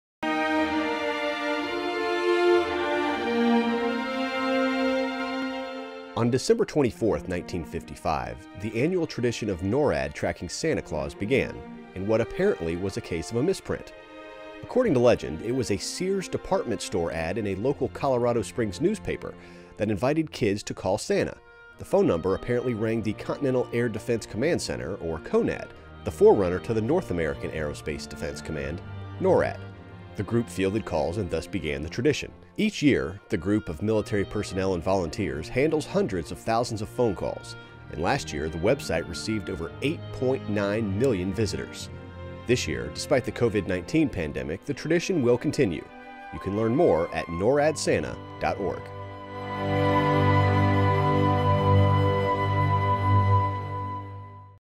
NORAD Santa Tracker Turns 65
On December 24th 1955, the annual tradition of NORAD tracking Santa Claus began.  This year despite the COVID-19 pandemic the tradition will continue.